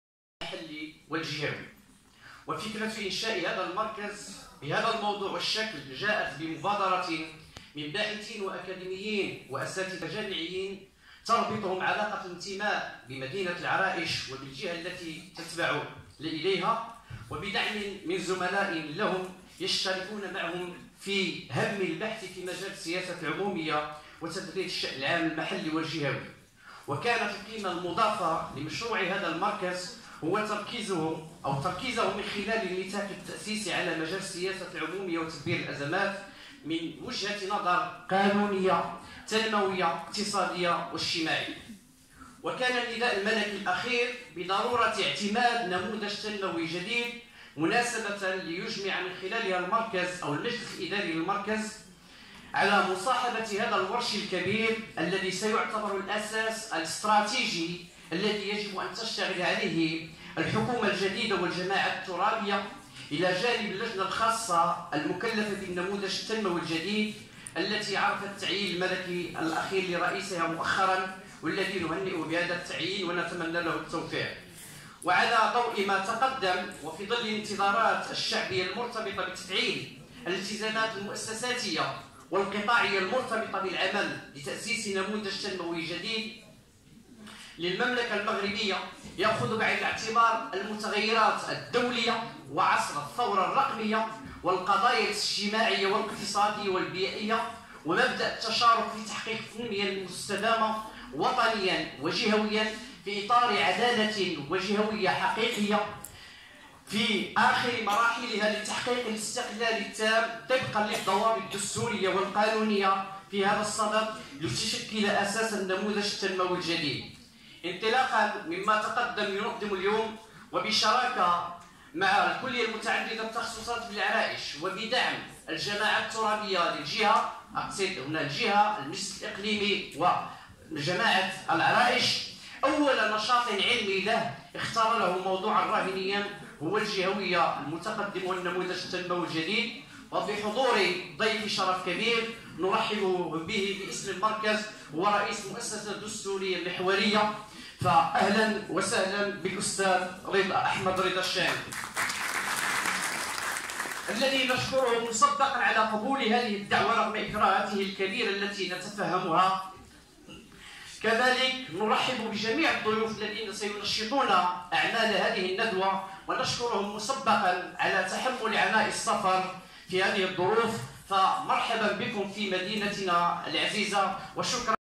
رئيس المركز المغربي للسياسات العمومية وتدبير الأزمات عبد الرحيم المودن يفتتح ندوة النموذج التنموي
رئيس المركز المغربي للسياسات العمومية وتدبير الأزمات عبد الرحيم المودن يفتتح الندوة الوطنية بالعرائش حول “الجهوية المتقدمة والنموذج التنموي الجديد بالكلية المتعددة التخصصات بالعرائش